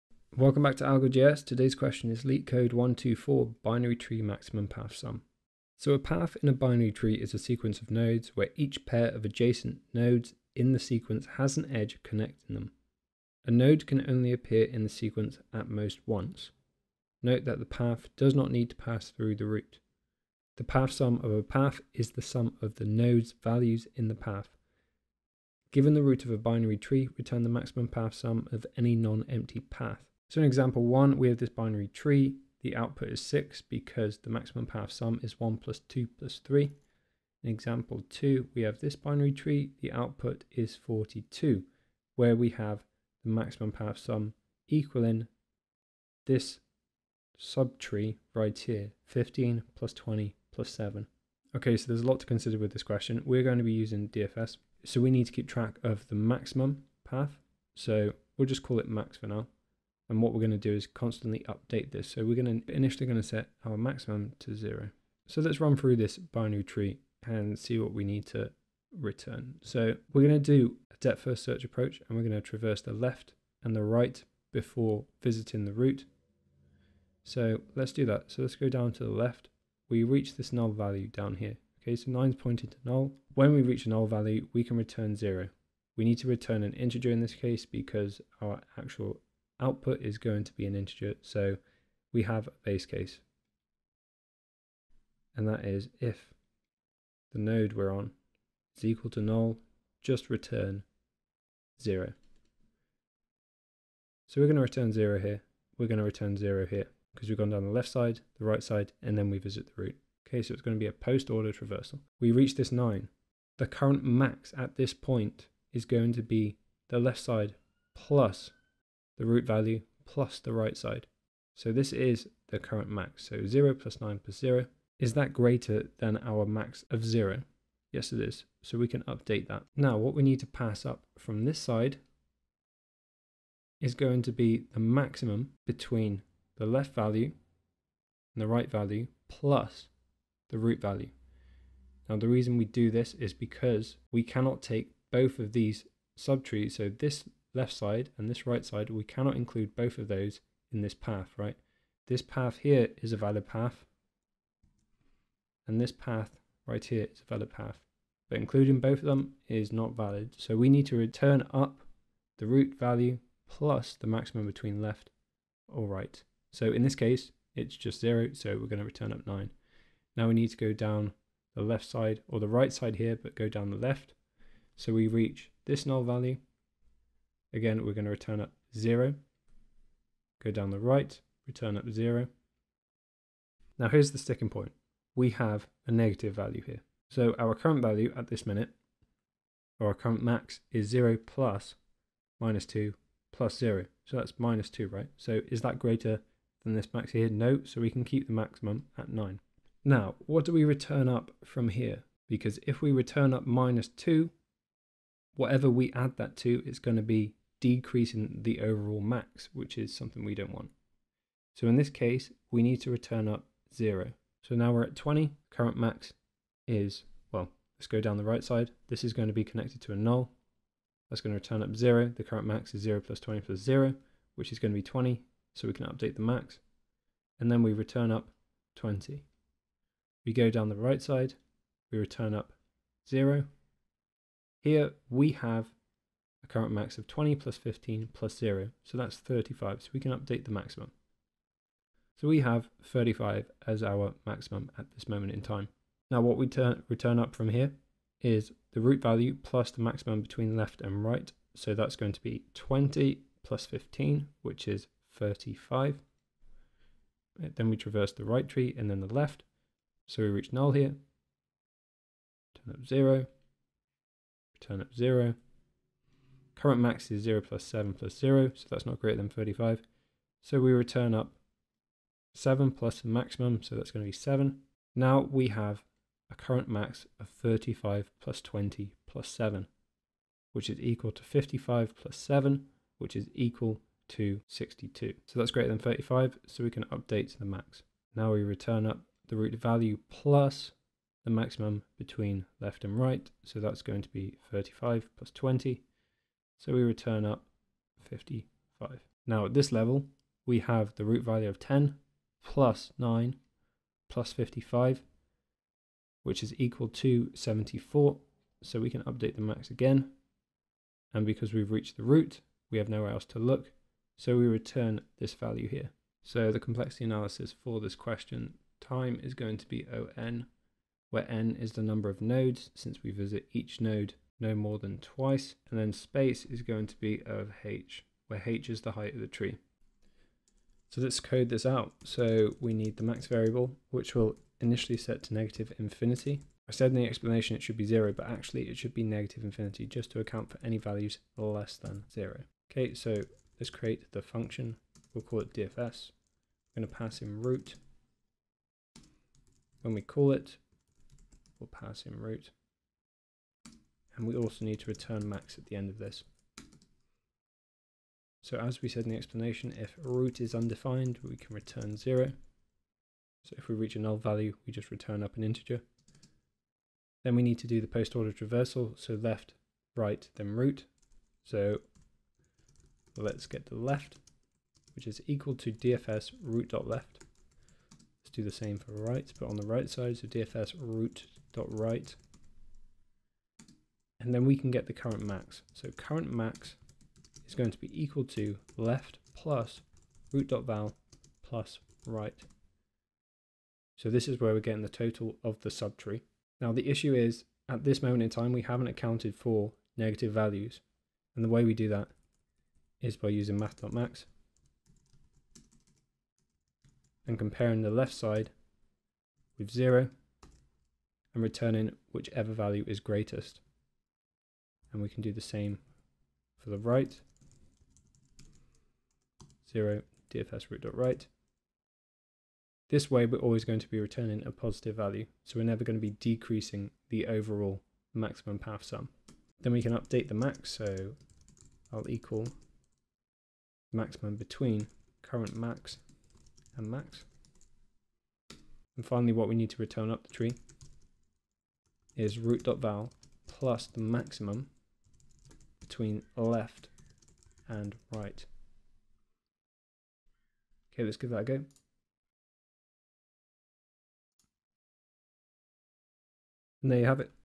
Binary Tree Maximum Path Sum - LeetCode 124 - JavaScript
Step by step walk through of the solution to the popular Amazon coding interview question, Binary Tree Maximum Path Sum.

LeetCode 124

JavaScript

0:00 Intro
0:51 Explanation
5:30 Code